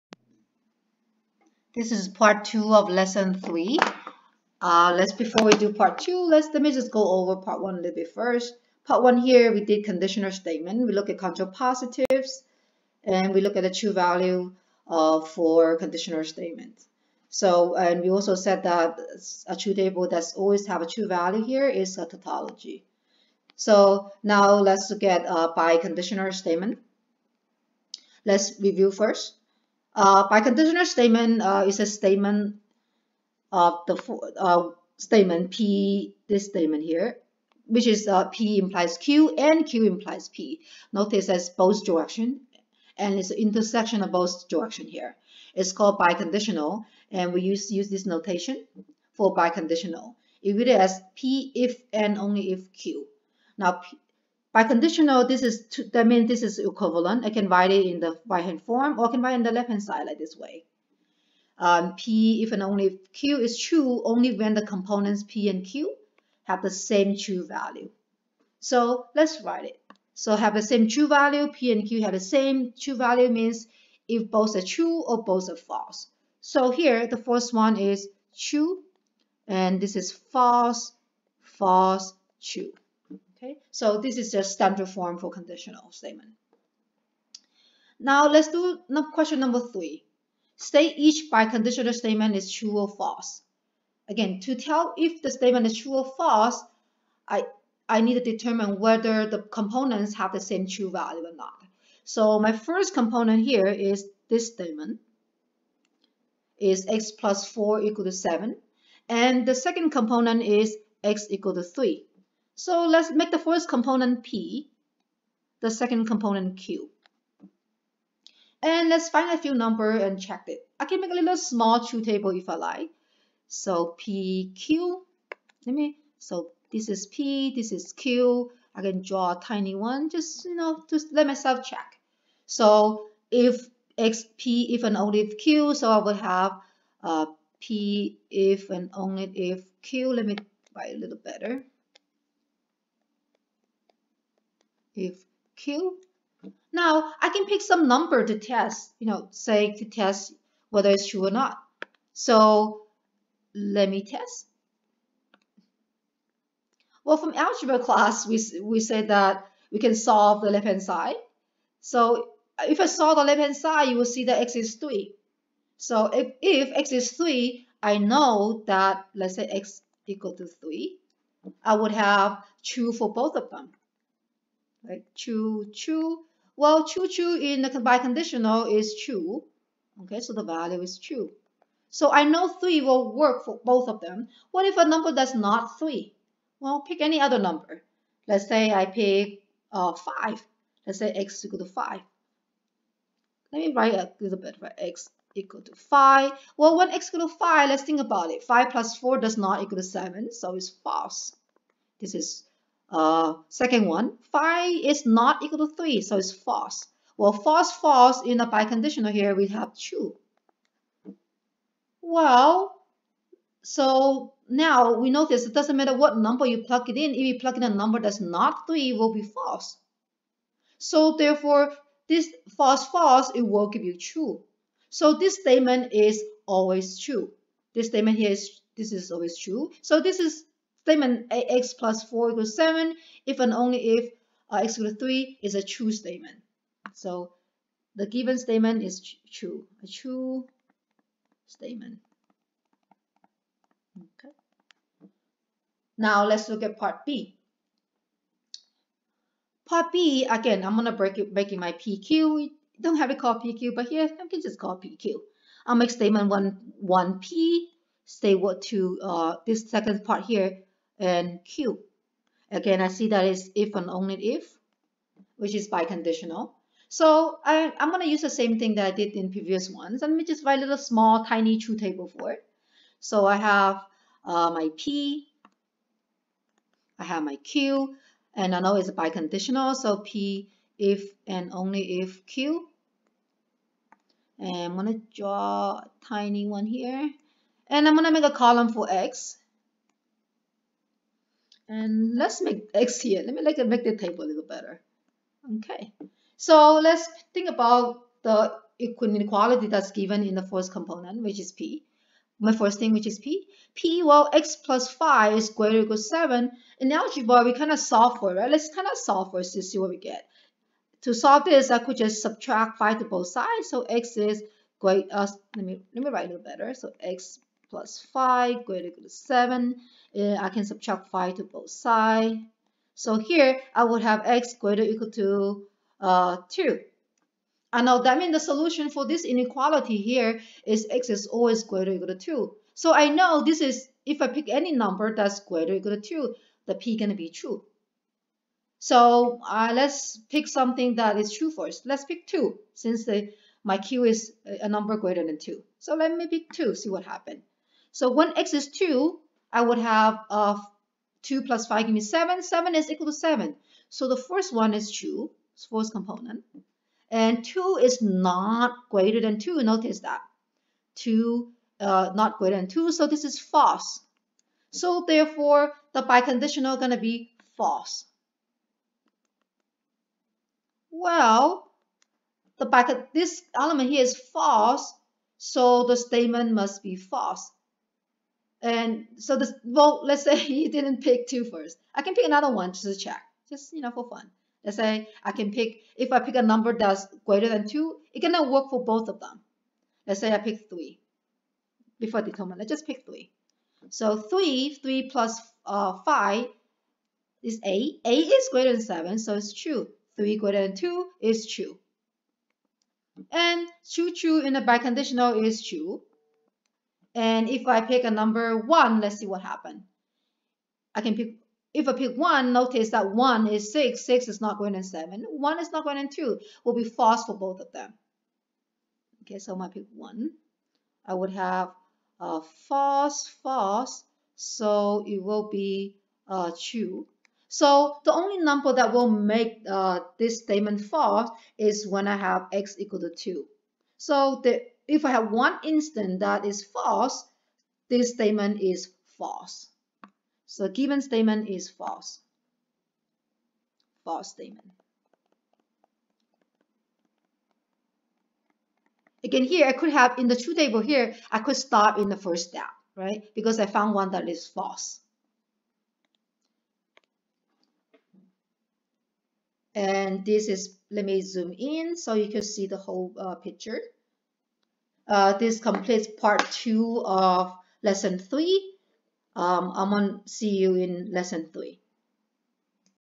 Lesson 3 Part 2 Biconditional Statements
Topics of this lesson includes review of biconditional statement and determine whether a biconditional is true or false.